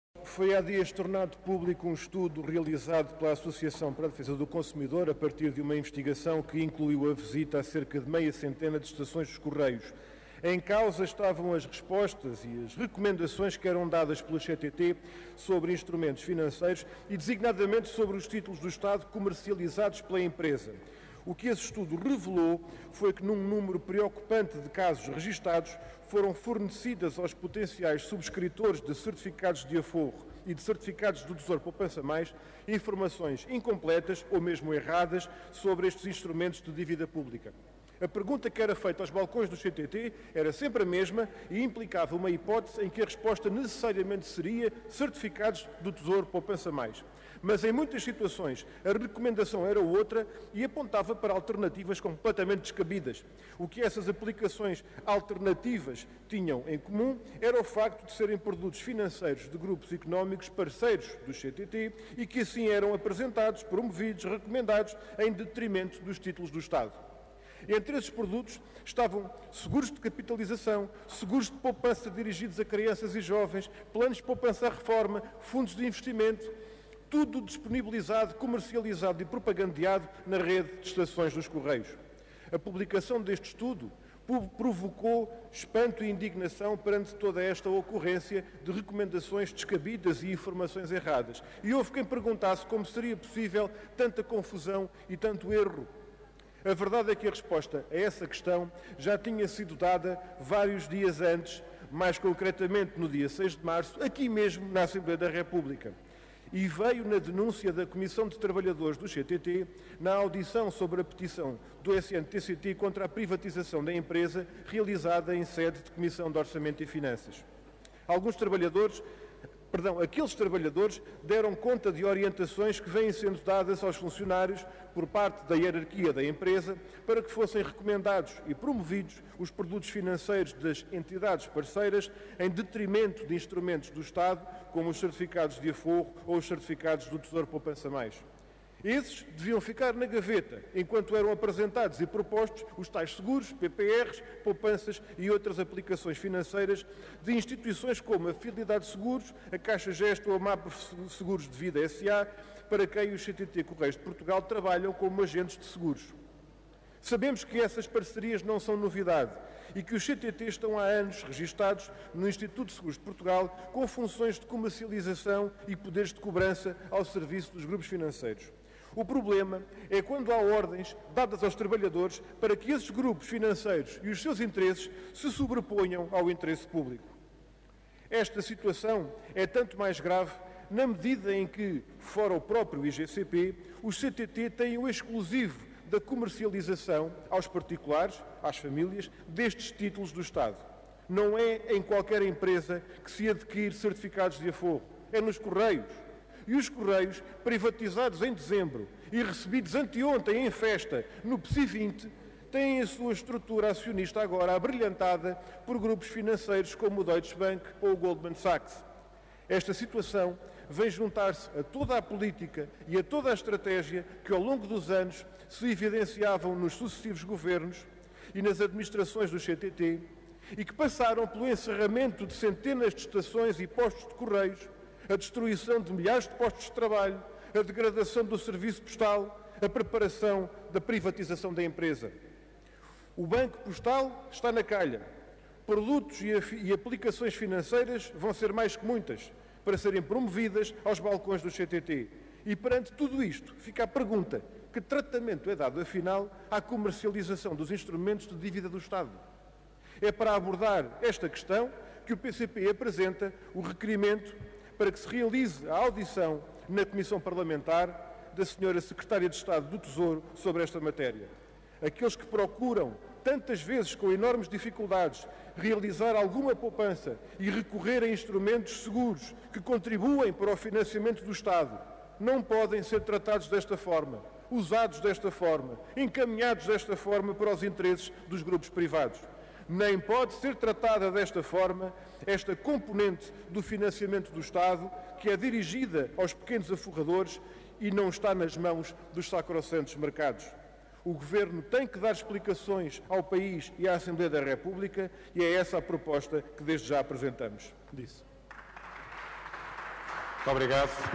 Os instrumentos de dívida na gaveta dos CTT
Na declaração política que o PCP levou hoje à tribuna da Assembleia da República, Bruno Dias afirmou que aqueles que procuram, tantas vezes com enormes dificuldades, realizar alguma poupança e recorrer a instrumentos seguros, que contribuem para o financiamento do Estado, não podem ser tratados desta forma, usados desta forma, encaminhados para os interesses dos grupos privados, nem pode ser tratada desta forma esta componente do financiamento do Estado, que é dirigida aos pequenos aforradores e não está nas mãos dos sacrossantos "mercados".